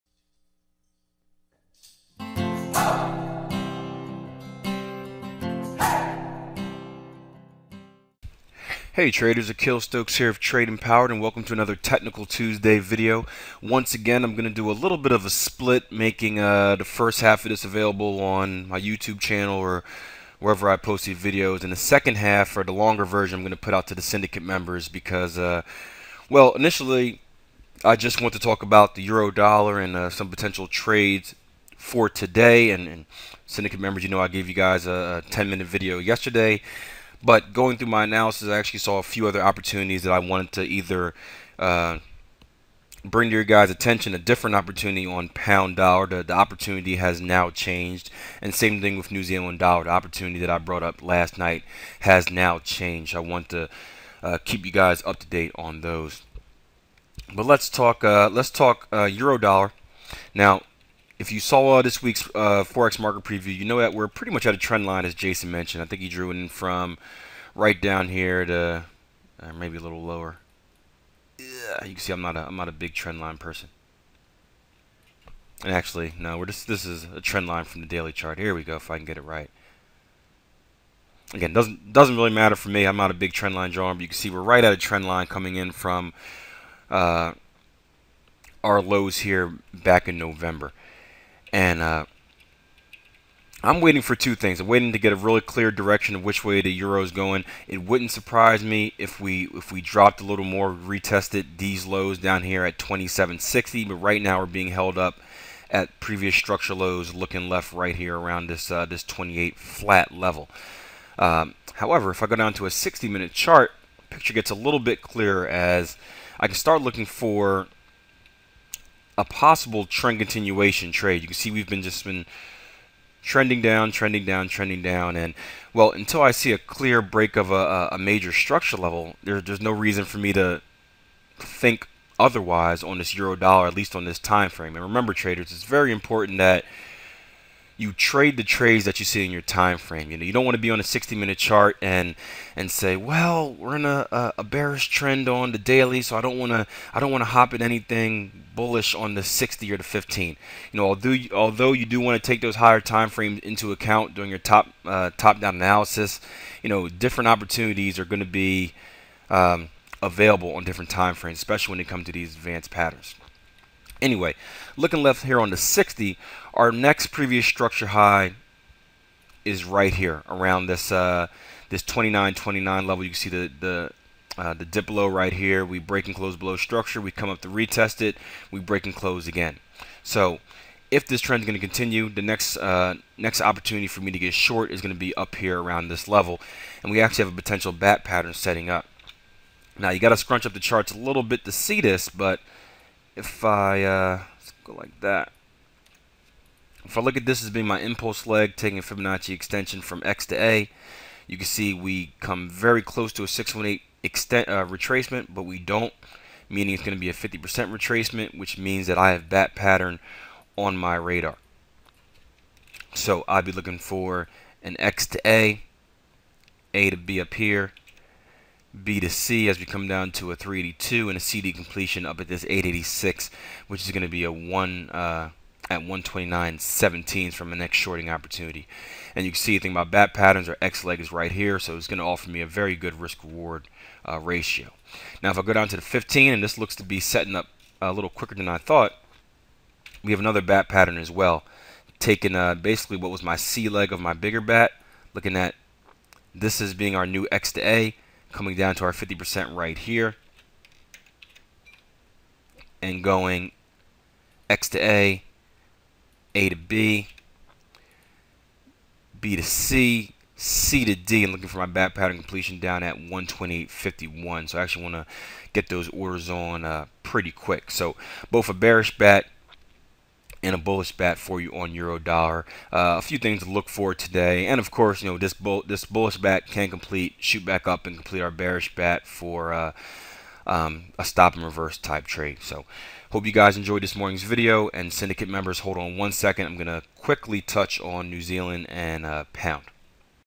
Forex Trading: What's in store for the Euro today
Forex trading using Advanced Pattern Recognition, Fibonacci Ratios & Structure Analysis.